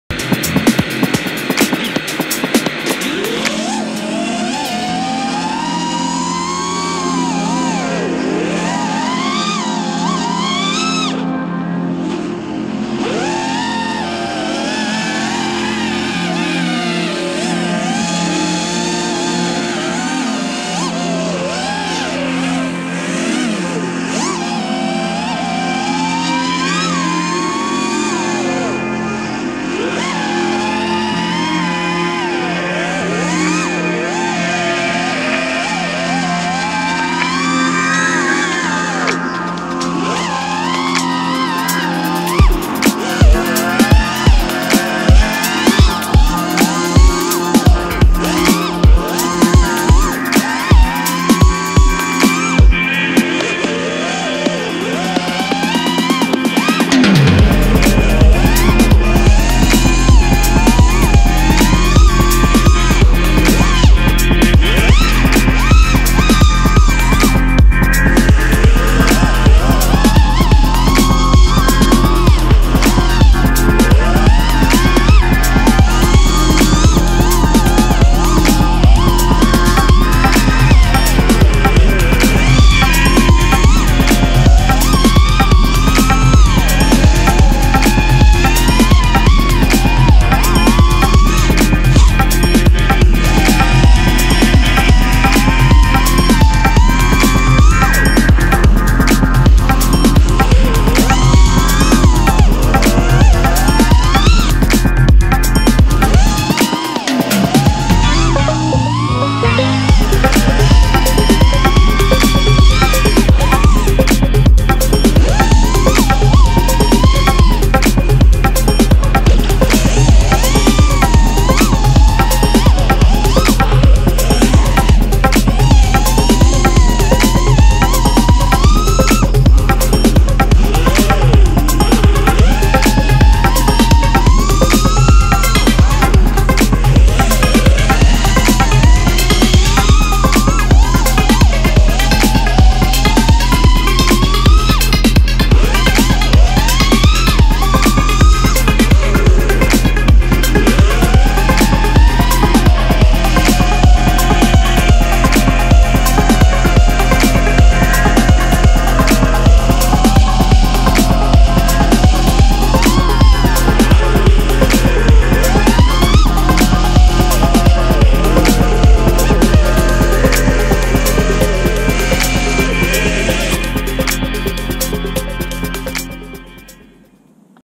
1 Pack Rip With 5" Ascent
This quad feels locked in a ready to go. New arm struts are super durable. Came back in 1 piece.